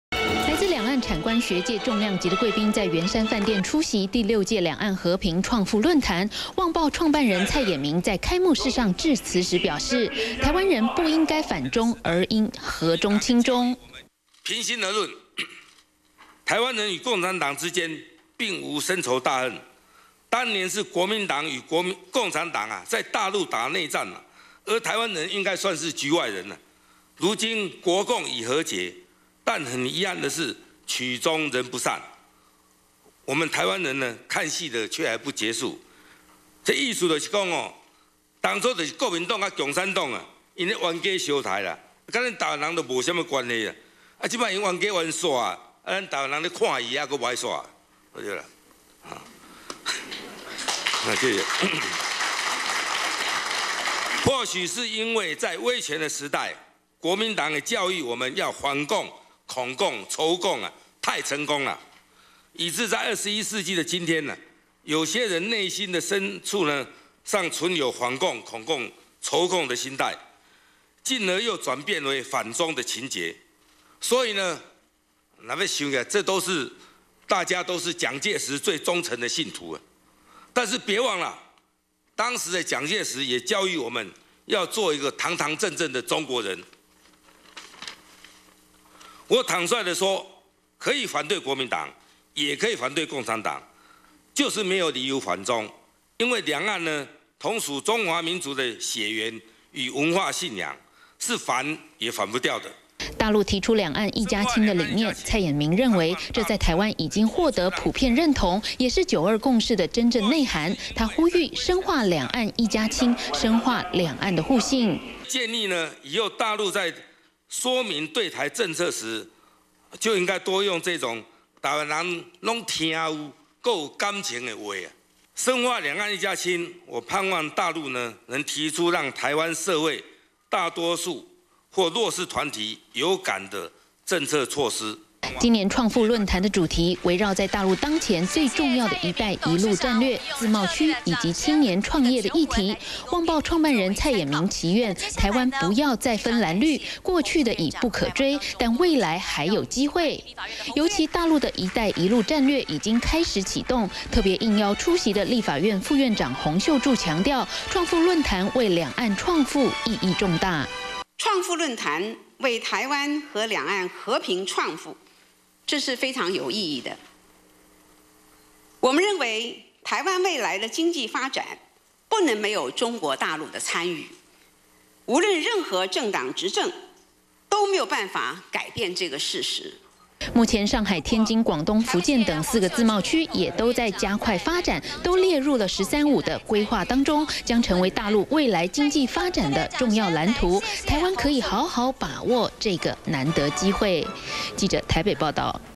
20150807中天新聞 旺報創富論壇 創辦人蔡衍明致詞
第六屆兩岸和平創富論壇，七號上午在圓山飯店登場，這一屆的主題聚焦在大陸一帶一路的戰略，以及自貿區和青年創業的議題，旺報創辦人蔡衍明致辭時也呼籲，台灣人不應反中，要深化「兩岸一家親及兩岸的互信」，共創雙贏。